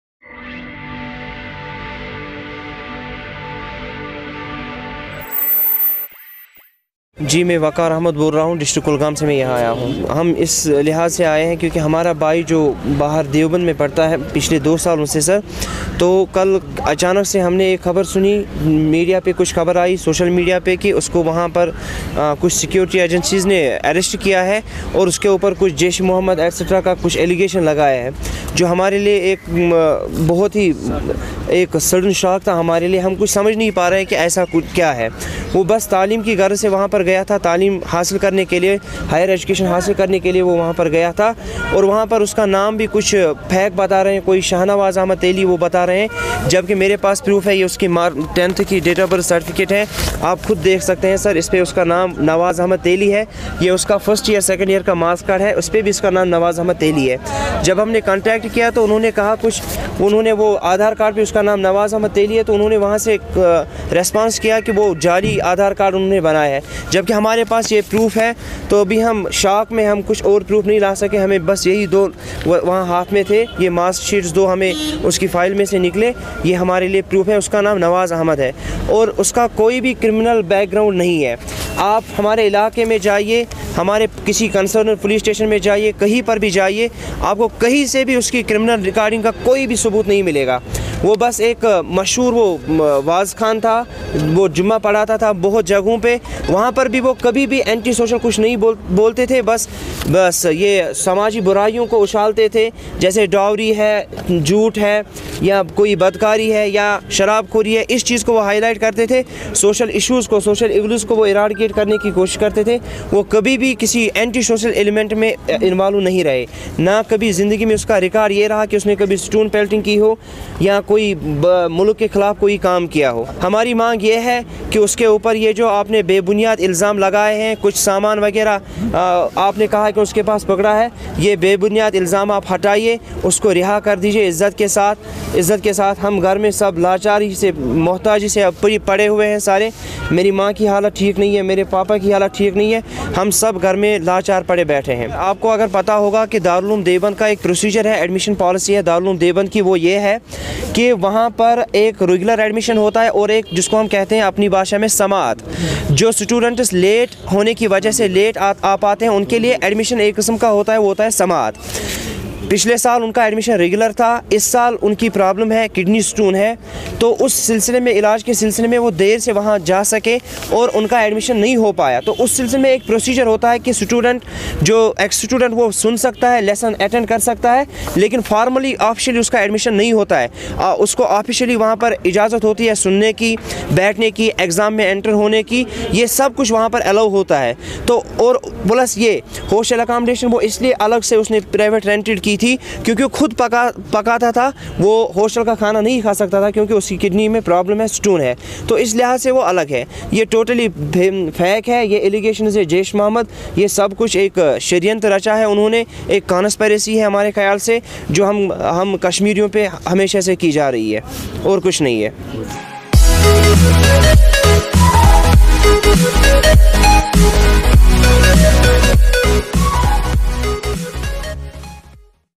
Family members of Kulgam youth arrested in Deoband hold protest, demand his release | GNN News
Uttar Pradesh police had on Friday claimed that Shahnawaz Ahmad Teli and another youth, identified as Aqib Ahmad Malik of Pulwama, were arrested in Deoband yesterday.
The family members of a youth arrested in Deoband town of Saharanpur yesterday over his alleged links with militant groups staged a protest in Srinagar on Saturday to demand his release.
A police official said the duo was posing as students and recruiting for Jaish-e-Muhammad militant outfit.
However, the family of Teli trashed the police claims, saying he was being framed under a false name.
Displaying the marks sheets in which he was named as Nawaz Ahmad Teli, his uncle said: “Police is making his case on a false name as his name is not Shahnawaz Ahmed Teli but Nawaz Ahmed Teli as written on documents. We are in pain and confusion as we don’t know what to do,” he said.
The protesting family members who had come all the way from Kulgam in south Kashmir to Press Enclave in Srinagar demanded his immediate release.
“My brother had gone there (Deoband) for religious education and he is innocent, we demand his release,” said Waqar Ahmed Teli.